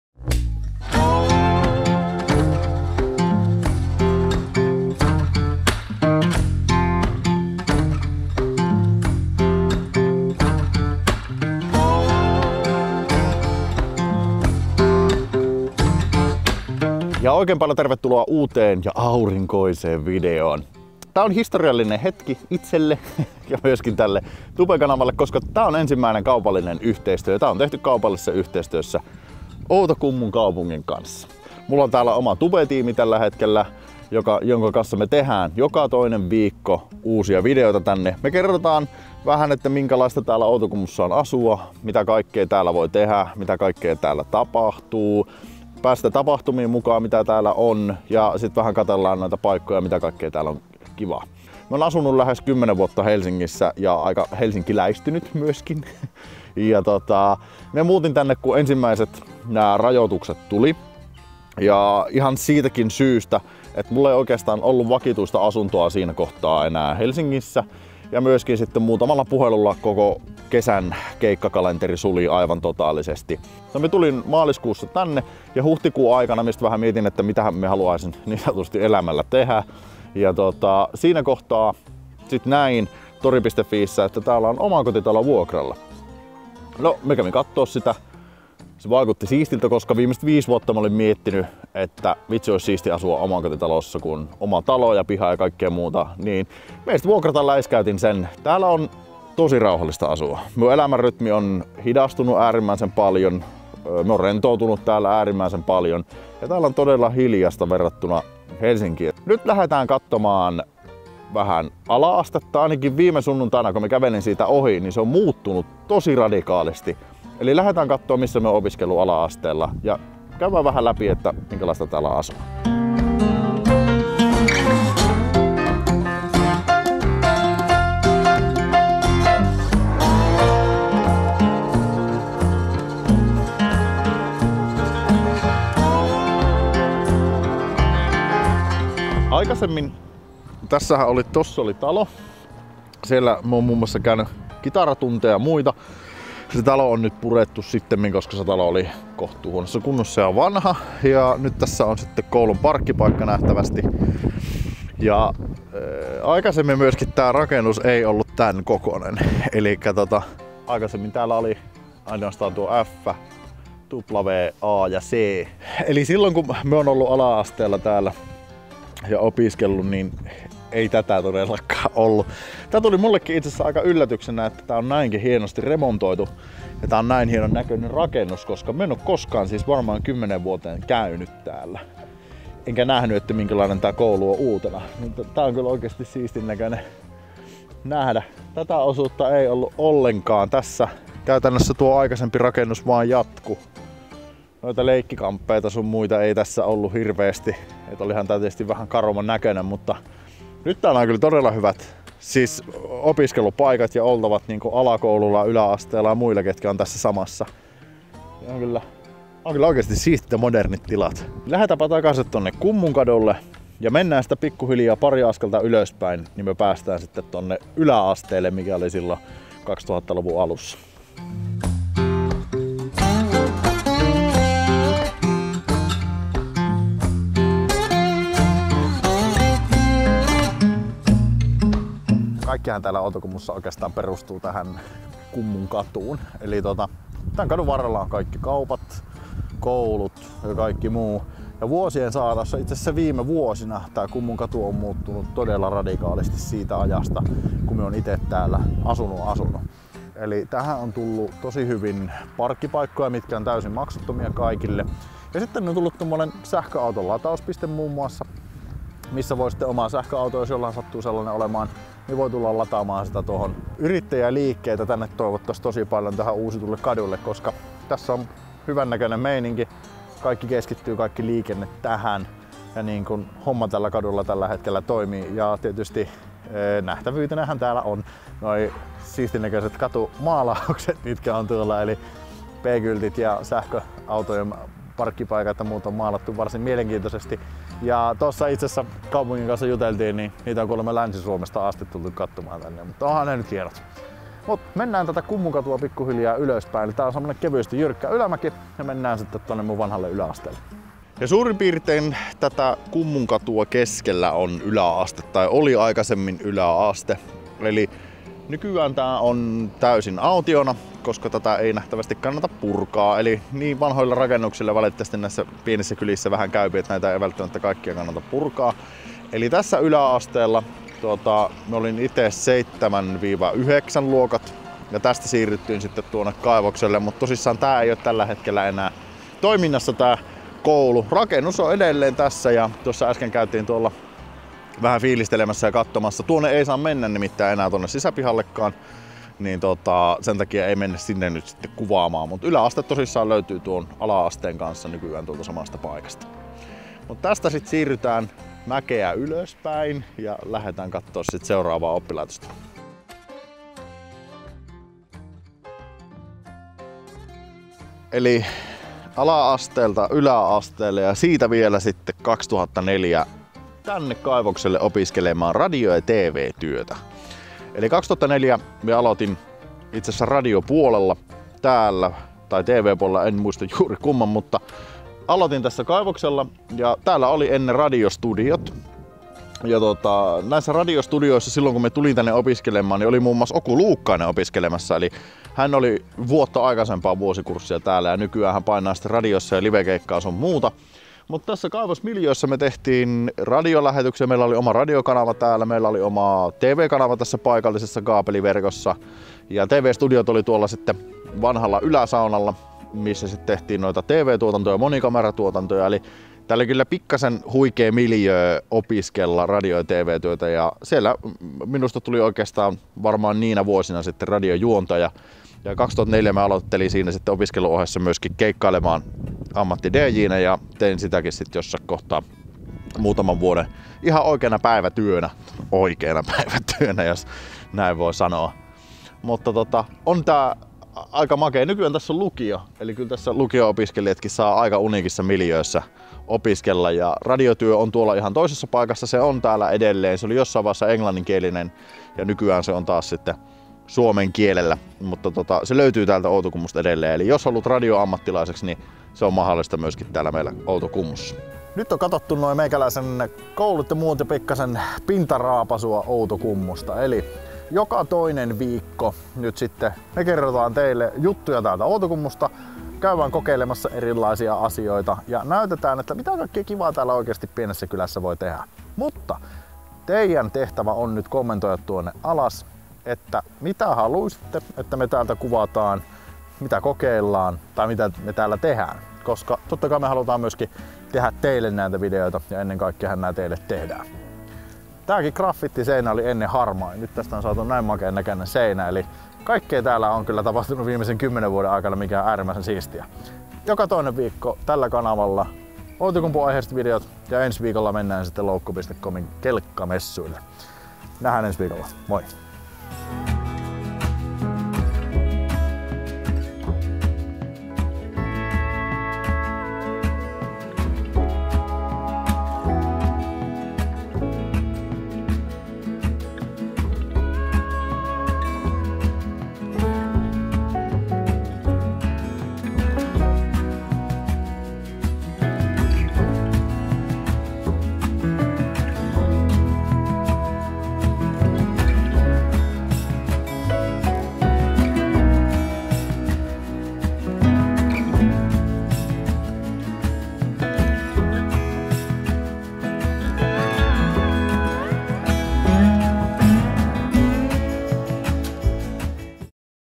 It's My Outokumpu - Uusi Sarja Alkaa! Mistä on kyse?
Nyt Alkaa uusi It's My Outokumpu - tubesarja! Tsekkaa videosta mistä on kyse ja ole kuulolla vähintään joka toinen viikko ;)

Ota samalla somet seurantaan!
Facebook: It's Me Harry Motorsport